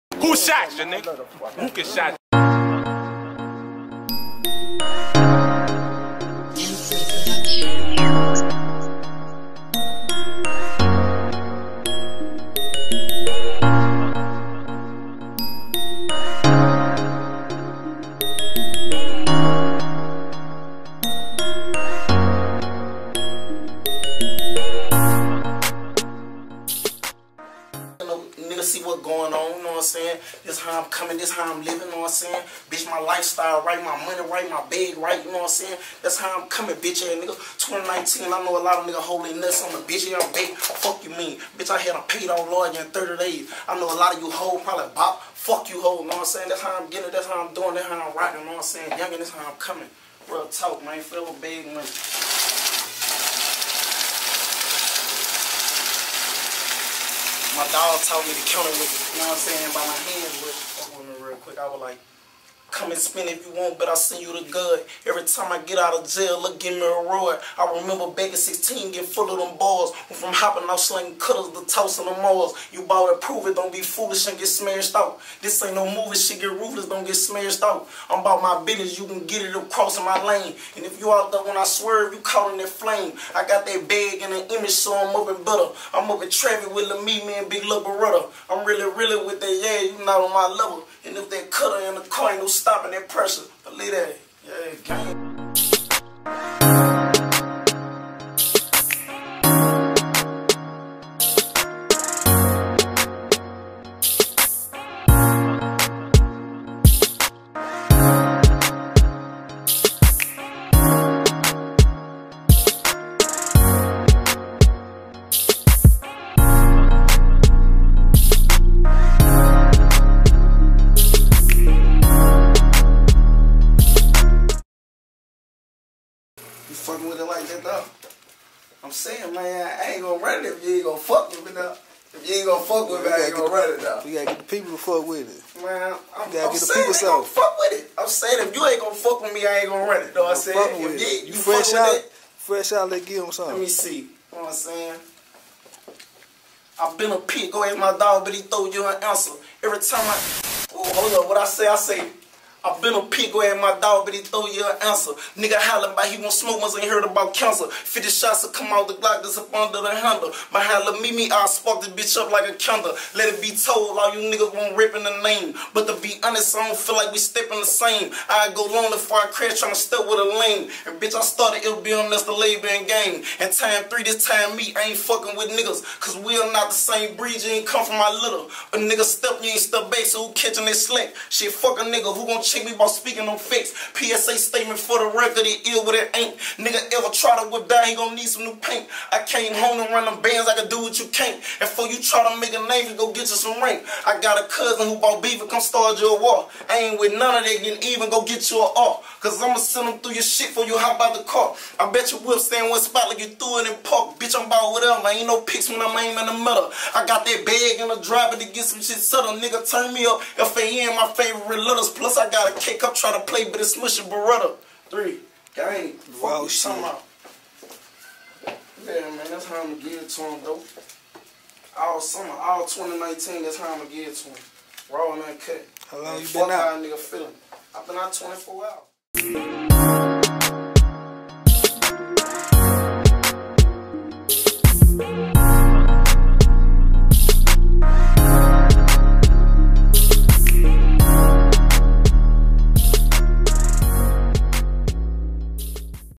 Fbm Youngin - Fresh Out Vlog
FBM Youngin first day out least then 24 hours running it up already